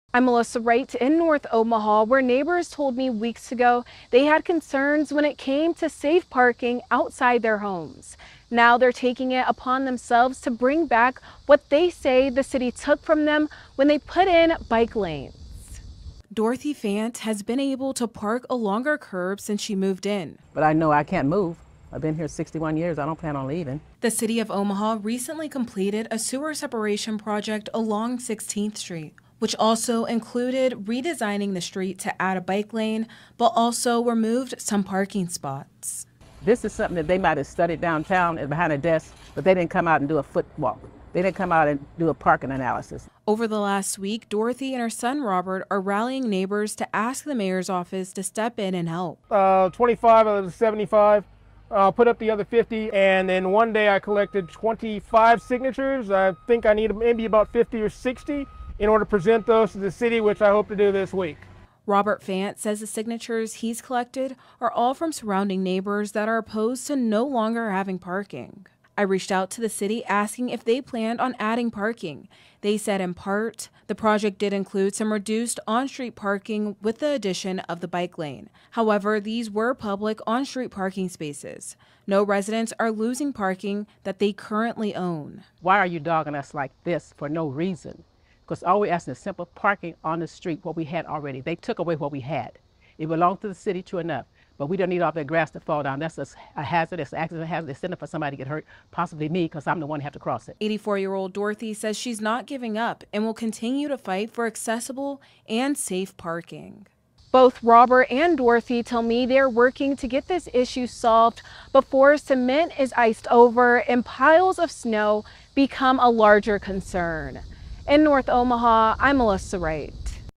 'I’ve been here 61 years and don't plan on leaving'
Neighbors along 16th street express concerns with new street design. They are now looking for a solution ahead of winter.